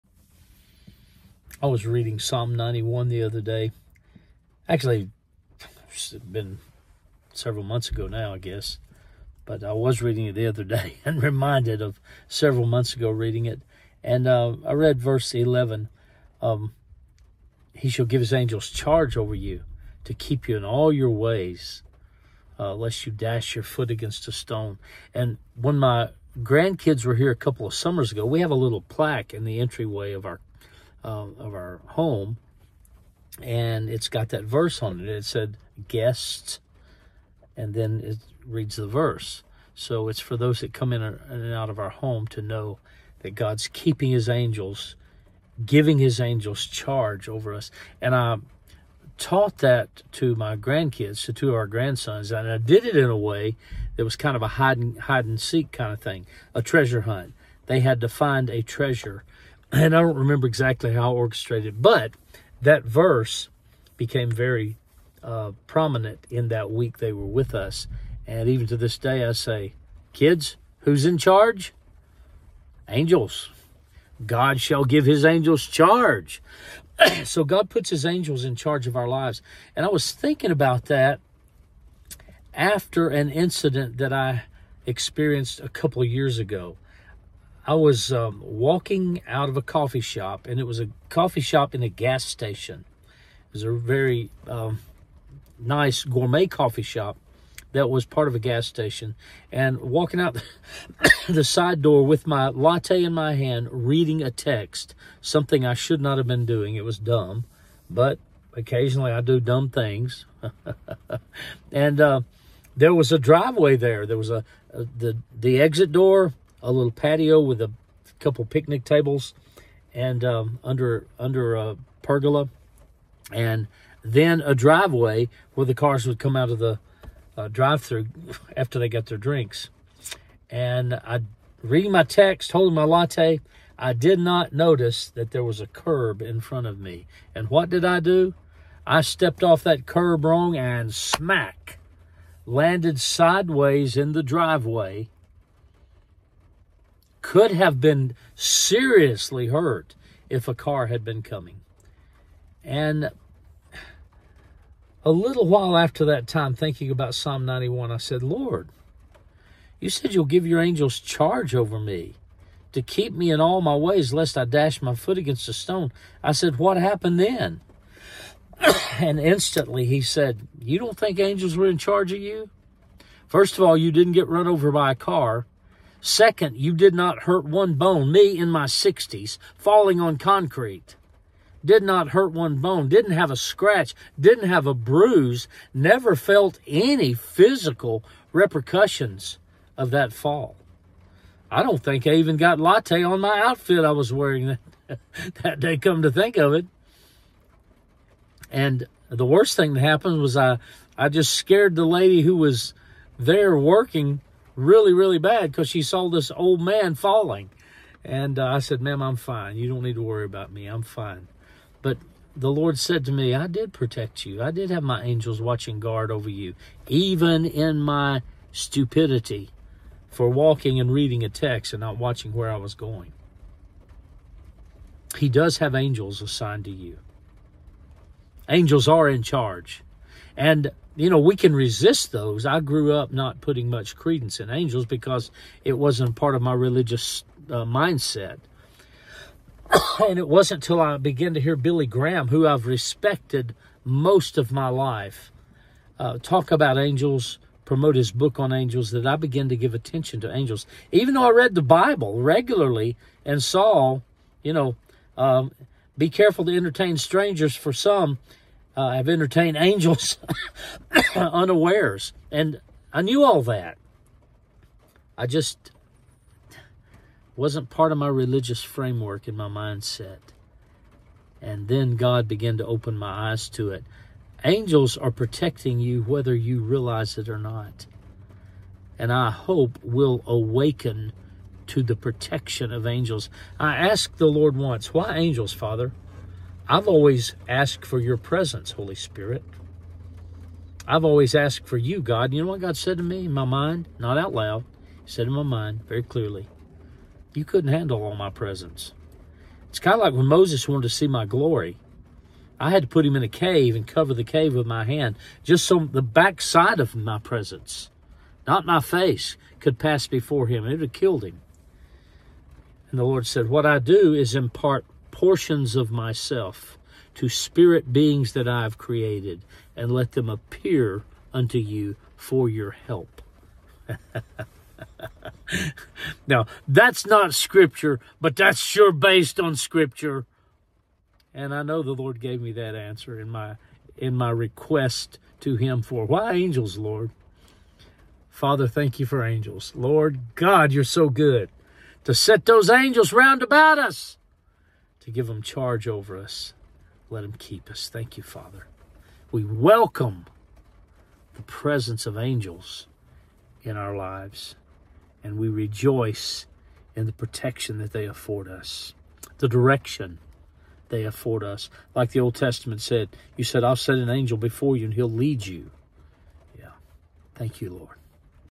Angels in charge/Psalm 91/Angels watching over you
“For he shall give his angels charge over thee, to keep thee in all thy ways. They shall bear thee up in their hands, lest thou dash thy foot against a stone.”

God assigns angels to our charge. He assigns angels for our protection. He assigns angels for our guidance and leadership.

God, dispatches angels who carry a part of his very presence to help us in areas where we are weak or otherwise need help. We could not handle all of God‘s presents at one time and so he piecemeals it out to us at need by angels .

Angels are watching over us whether or not we are aware of them.

Be careful to entertain strangers because they might be angels.

Hebrews 13:2  Be not forgetful to entertain strangers: for thereby some have entertained angels unawares.